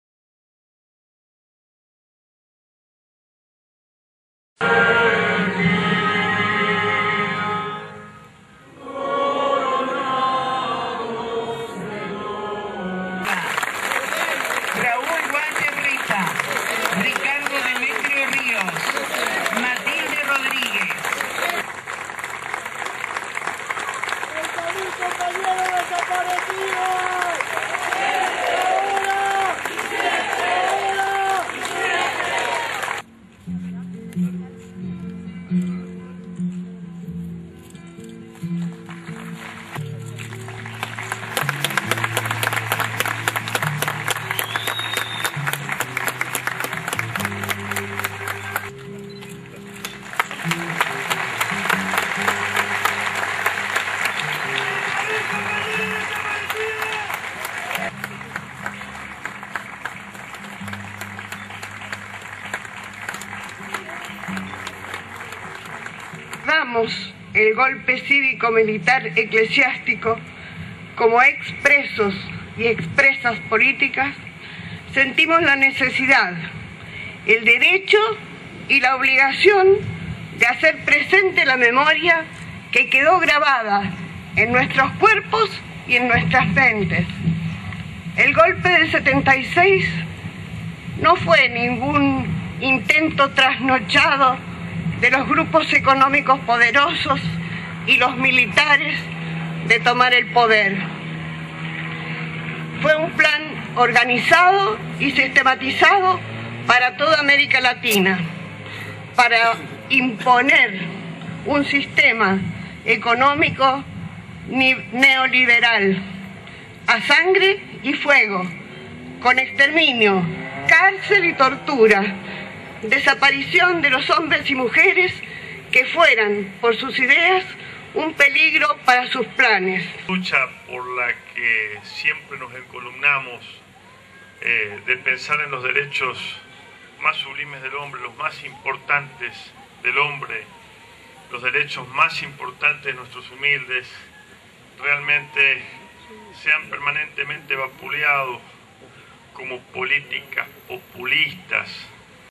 OTRA VEZ LA MUNICIPALIDAD PRESENTE
También este año, el Municipio de San Rafael organizó y realizó un Acto Homenaje por la Memoria, la Verdad y la Justicia